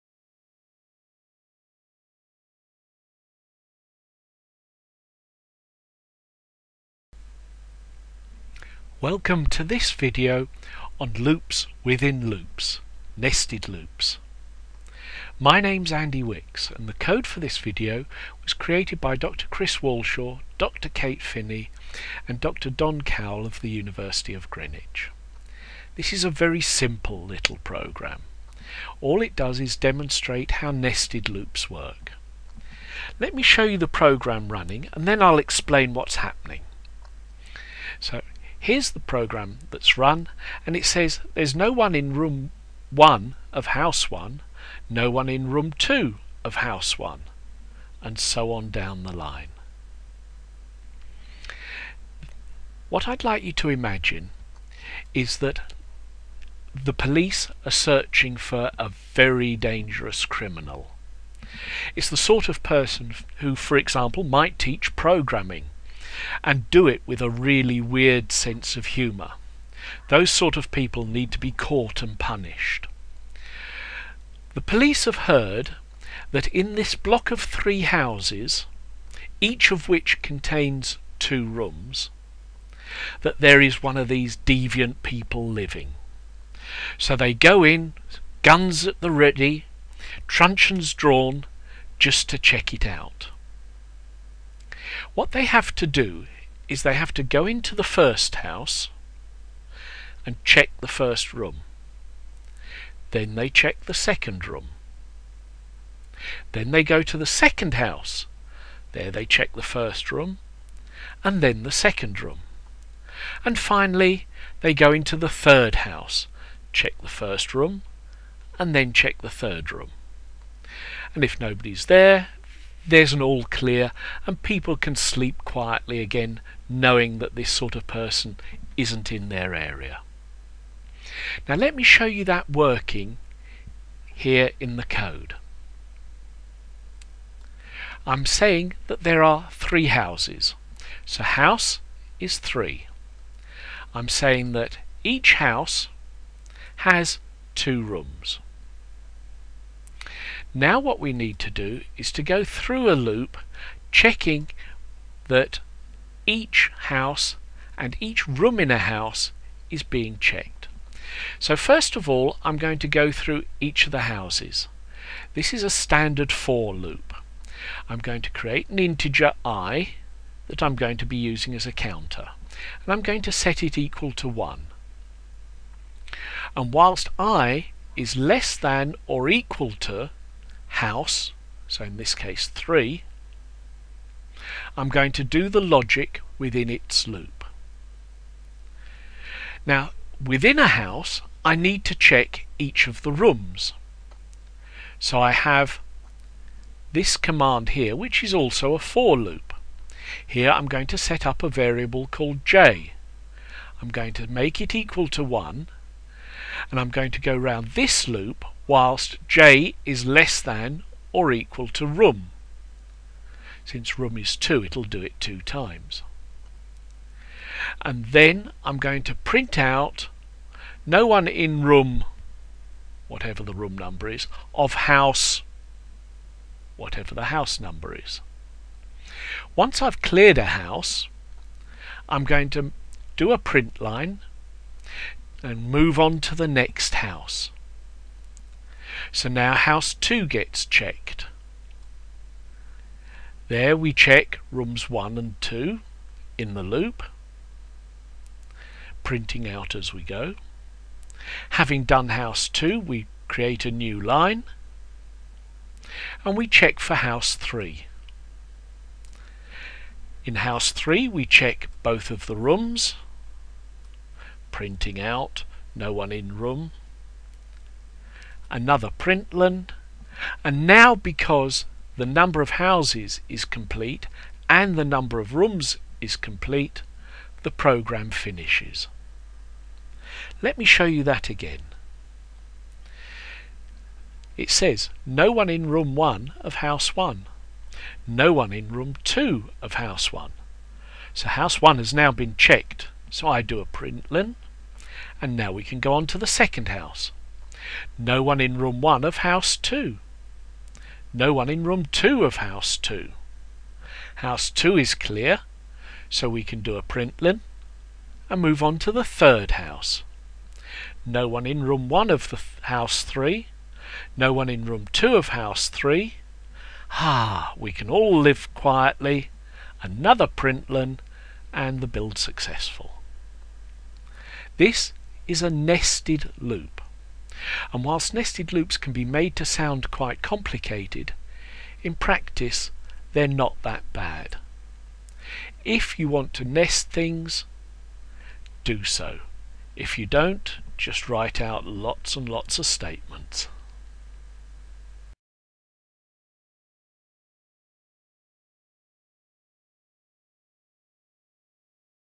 Java - Nested loops
This video explains how to use nested loops.  The example uses Java, but the same principles hold true in all other programming languages.  Problems sometimes need breaking down into parts that are repeated according to more than one criterion.